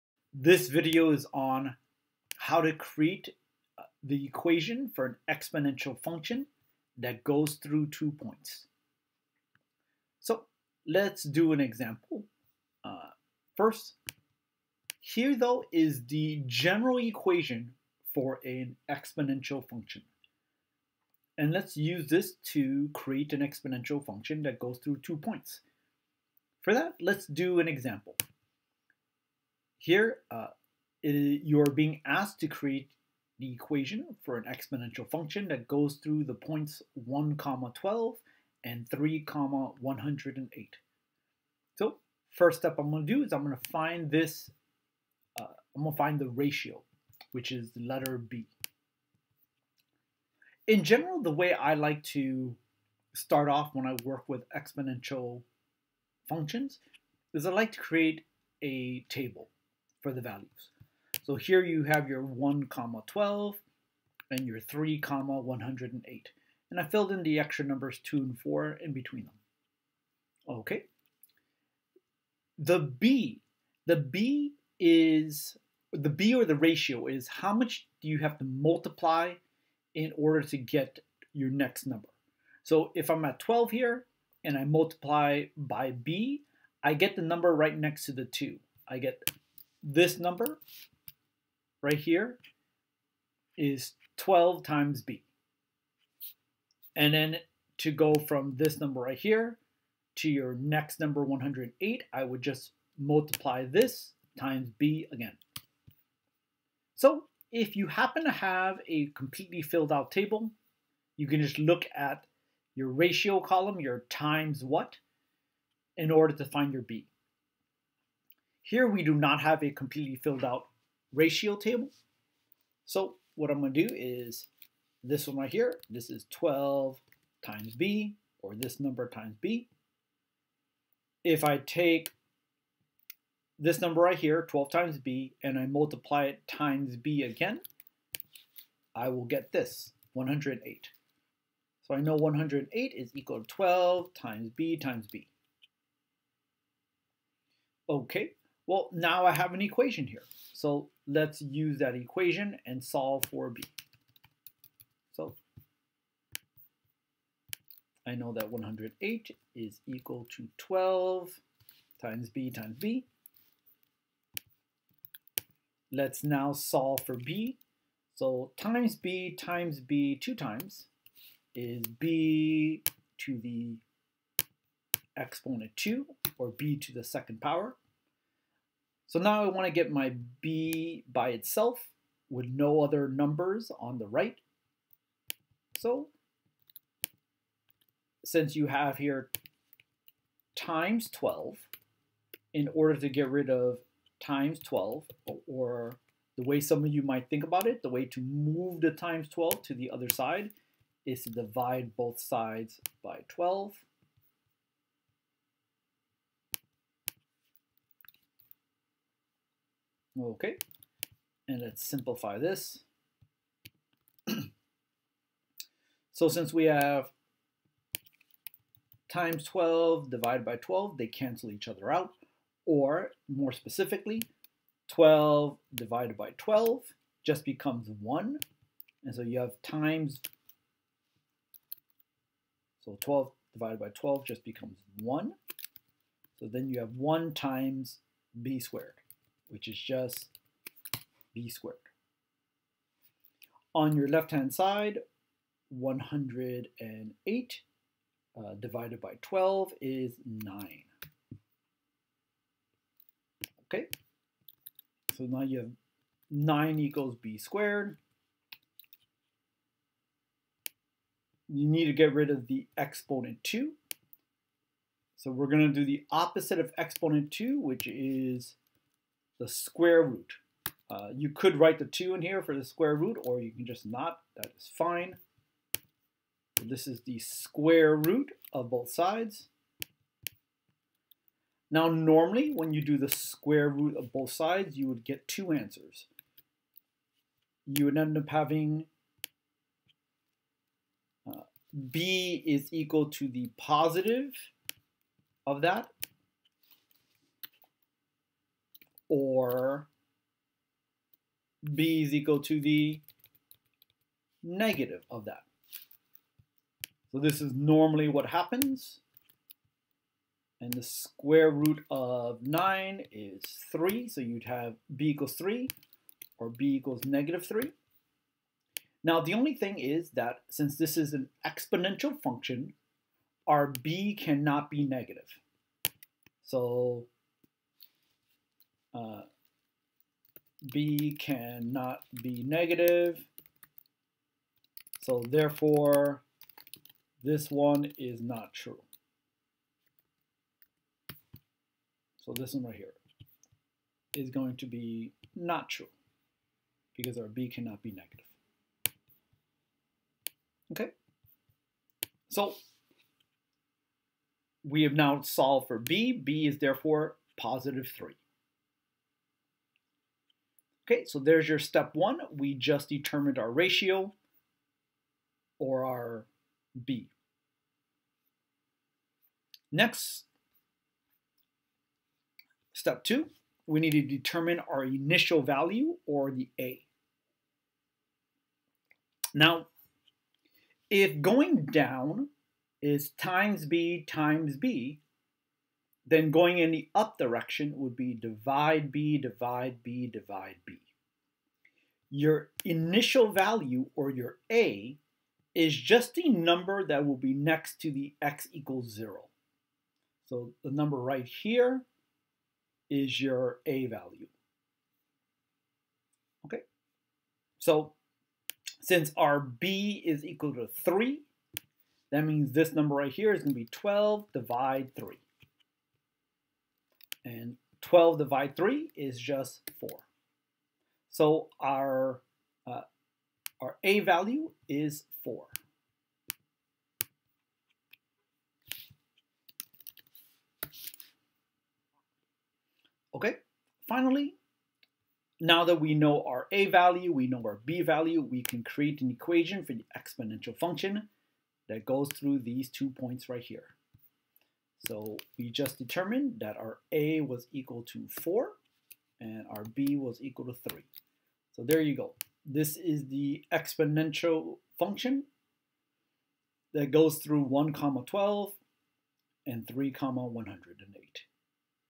Equation for exponential function y=A*B^x
How to create the equation for an exponential function that goes through 2 points.